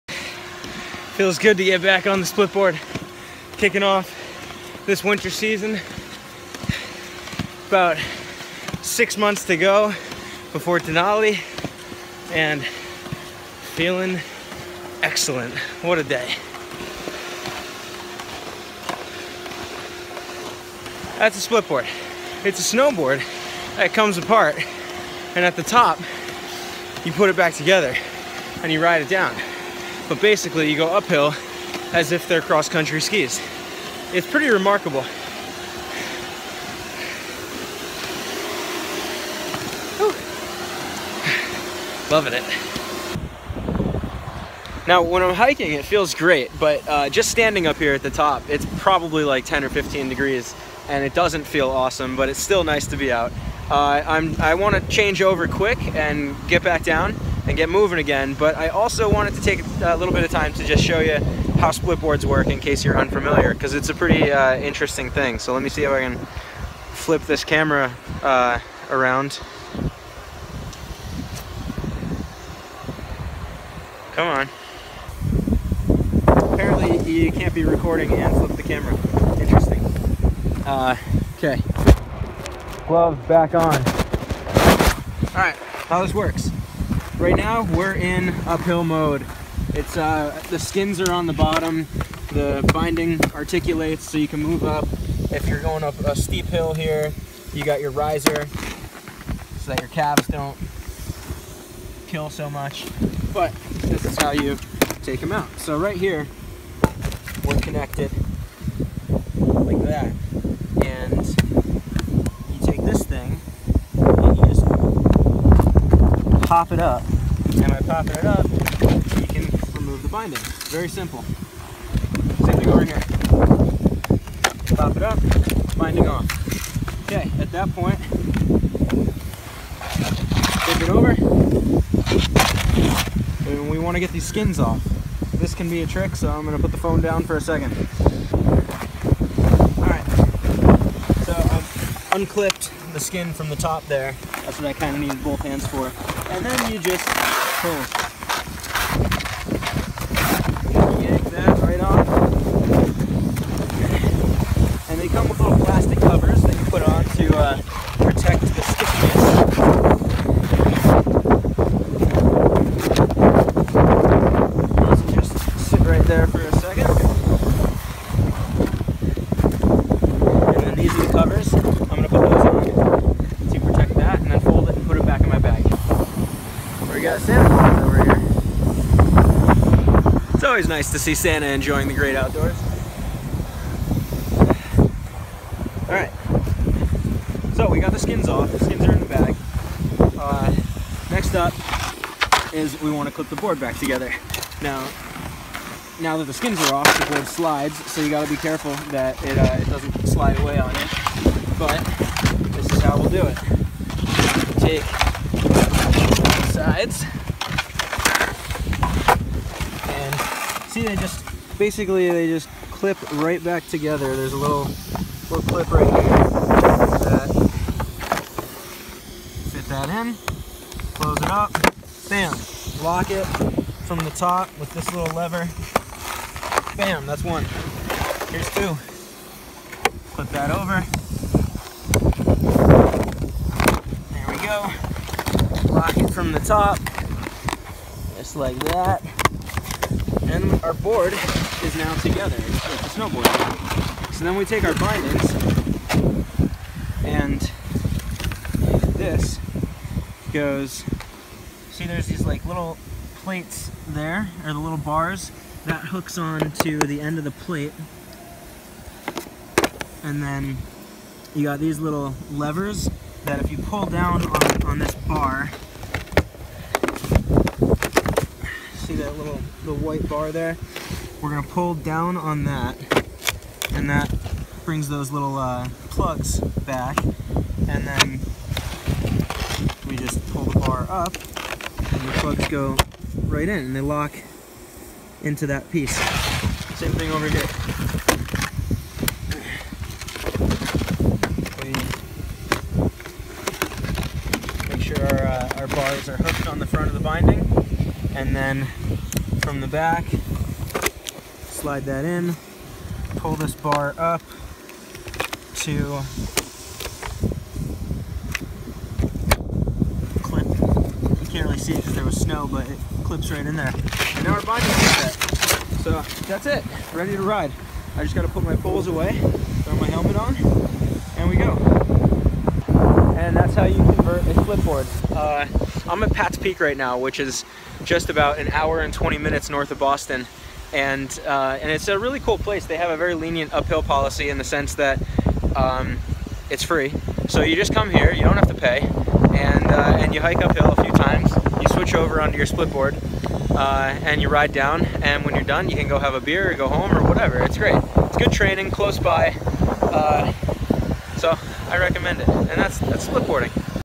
What is a splitboard and how does it work?
In my world, splitboarding is a pretty common thing. A lot of my friends do it, and I have for a few years now. But from the looks I still get out on the mountains, and many conversations I've had recently, I guess a lot of people have never heard of splitboarding, have never seen a splitboard, and/or just don't get the concept of it. This video is for all of those people, and maybe you!

Here's how you use a splitboard!